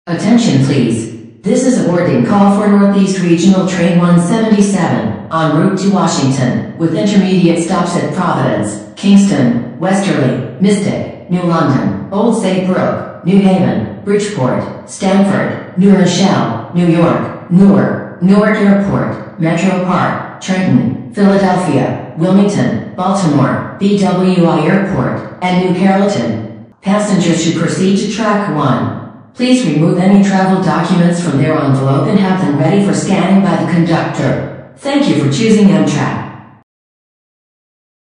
Amtrak Northeast Regional Train 177 to Washington Announcements at Route 128 Station
Station: Route 128/University Park Station
Location: 50 University Ave, Westwood, MA 02090